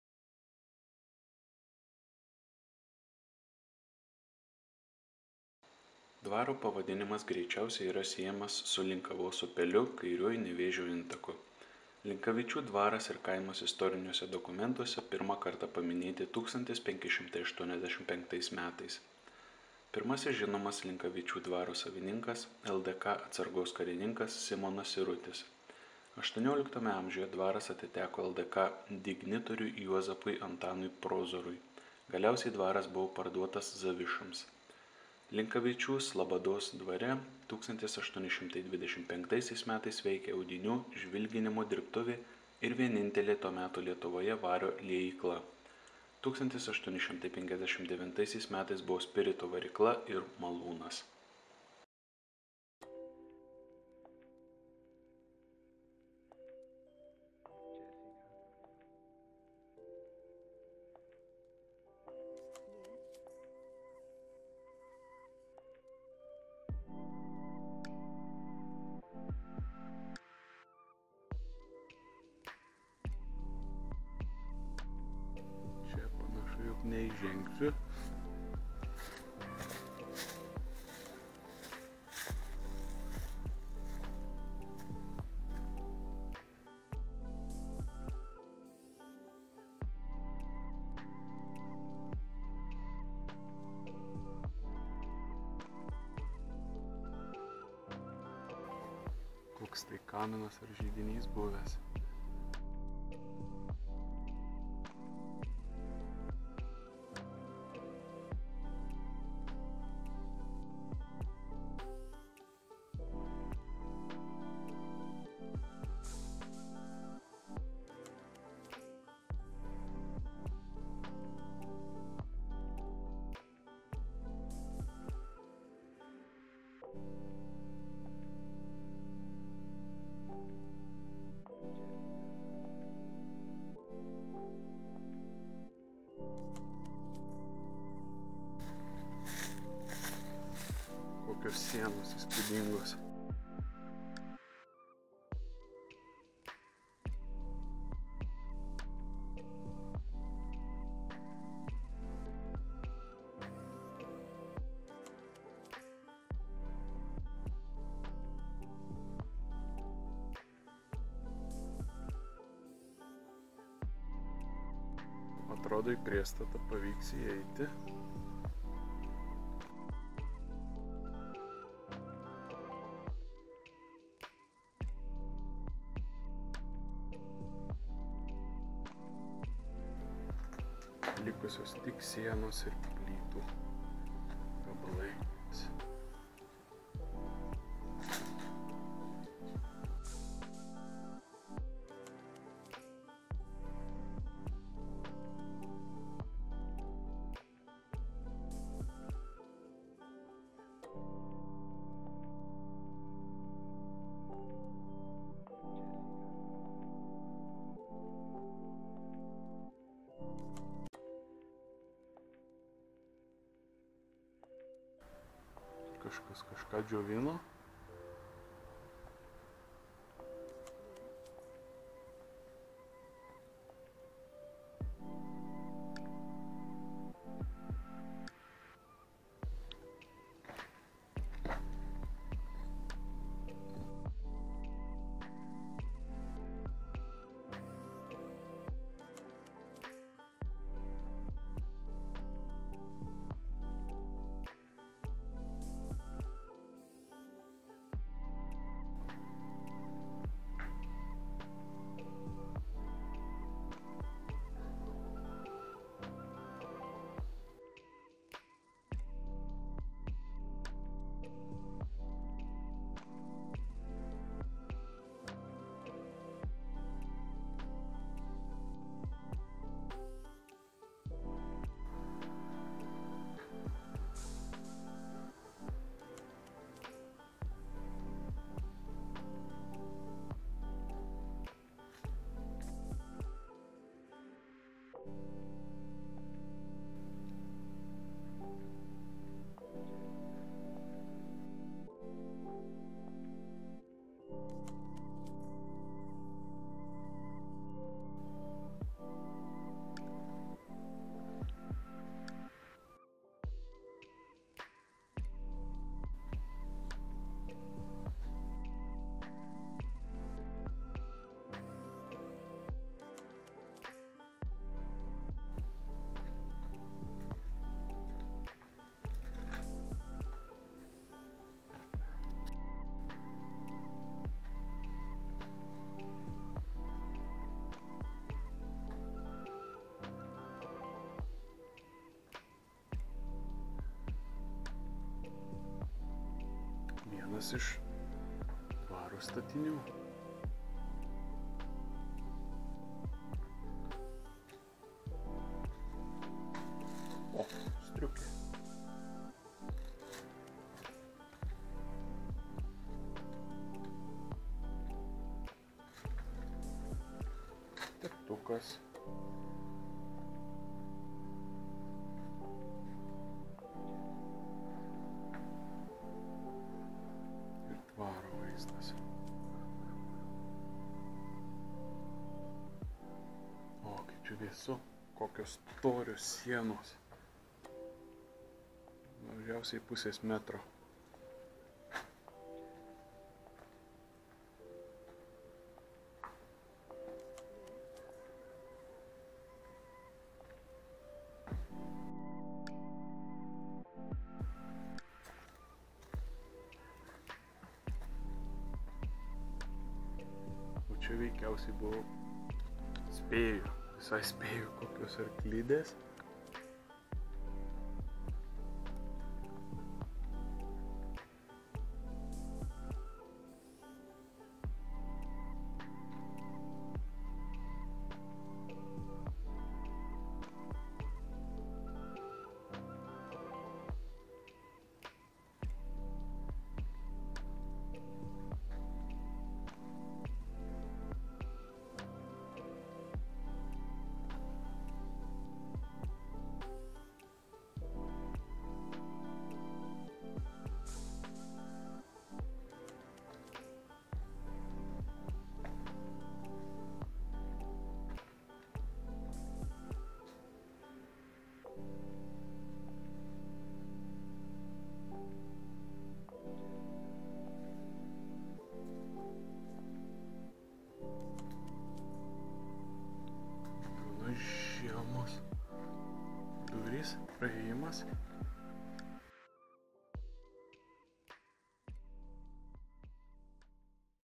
Linkavičių dvaro sodybos likučiai/ Remains of Linkavičiai manor
Šį kartą lankausi Linkavičių dvaro sodyboje.

Dvaro pavadinimas greičiausiai yra siejamas su Linkavos upeliu, kairiuoju Nevėžio intaku. Linkavičių dvaras ir kaimas istoriniuose dokumentuose pirmą kartą paminėti 1585 metais. Kiti XVI amžiaus šaltiniai mini Linkavičių palivarką. Pirmasis žinomas Linakvičių dvaro savininkas - LDK atsargos karininkas Simonas Sirutis, XVIII a. dvaras atiteko LDK dignitoriui Juozapui Antanui Prozorui, galiausiai dvaras buvo parduotas Zavišoms. Manoma, jog Linkavičių dvare 1873 m. gimė žymi Nepriklausomos Lietuvos visuomenės bei kultūros veikėja Felicija Povickaitė - Bortkevičienė. 1941 m. Paulina ir Povilas Zavišos buvo ištremti į Sibirą. Dvaro žemė paskutinį kartą padalinta 1944 m. Nuo to laiko ji buvo apleista, šiuo metu dalinai privatizuota.

Linkavičių-Slabados dvare 1825 m. veikė audinių žvilginimo dirbtuvė ir vienintelė to meto Lietuvoje vario liejykla, 1859 m. buvo spirito varykla ir malūnas.

Informacijos šaltinis: kpd.lt
Įspėjimas: statiniai, kuriuose (ar greta, kurių) lankausi, gali būti nestabilios konstrukcijos ir paveikti erozijos, juose ar greta jų lankytis gali būti pavojinga.